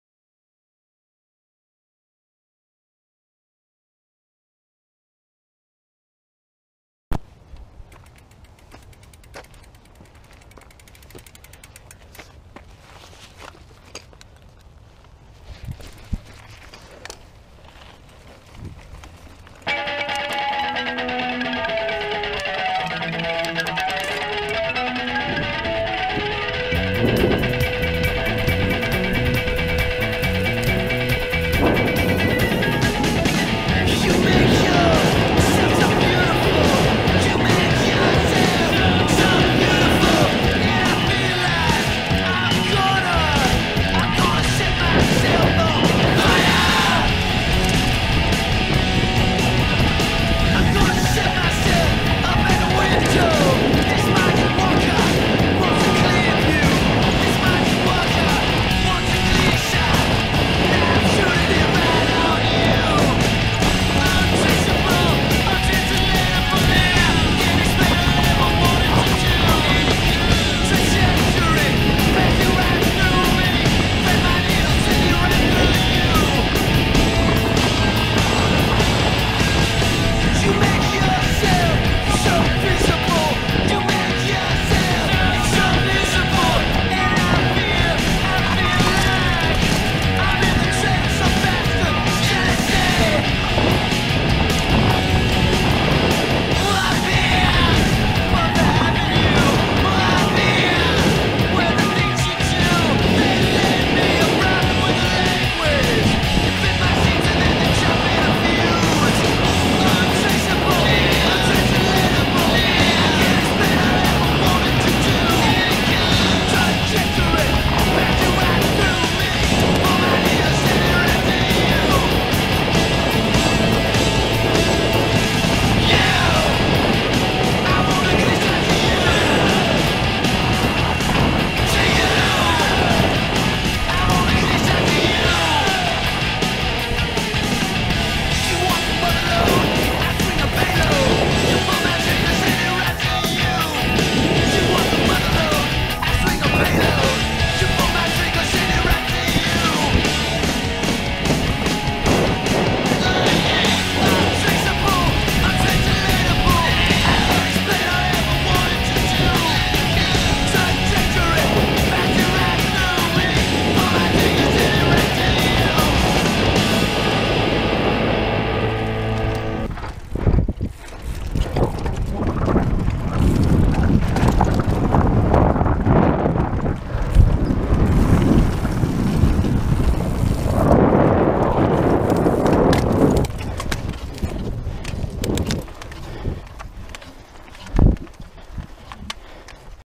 Santiago Oaks - Deer Downhill single track mountain bike trail
Santiago Oaks - Deer Trail.  Downhill single track mountain bike trail in Anaheim Hills, CA.

Strava
Deer - Downhill only 2:32